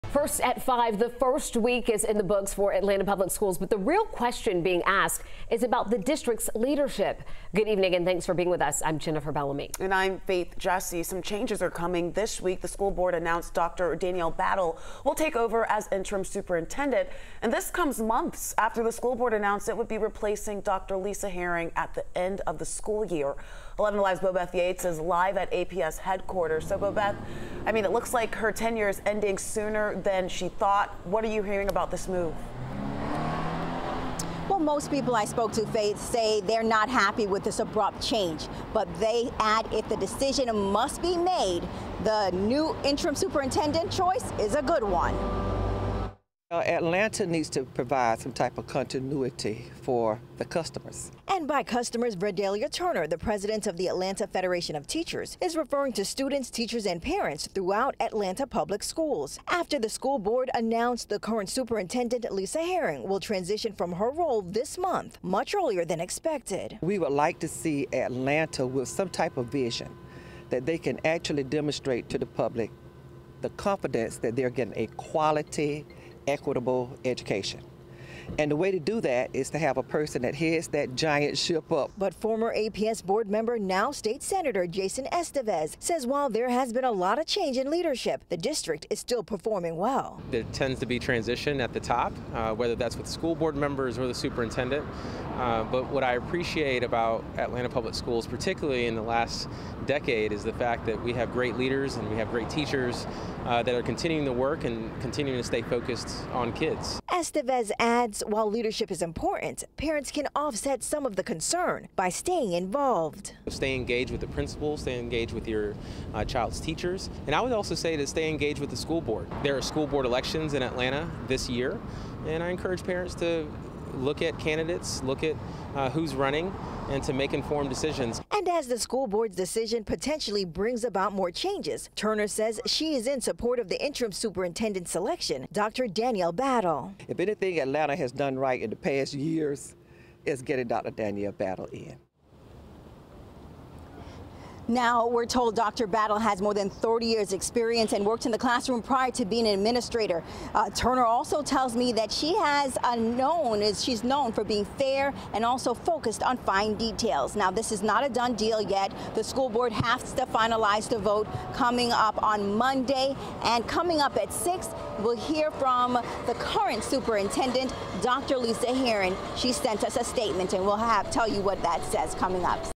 APS Interim Superintendent Dr. Danielle Battle | Reactions
Dr. Danielle Battle has been selected as the APS interim superintendent pending board approval on Monday, Aug. 7, according to the Atlanta Board of Education (ABOE).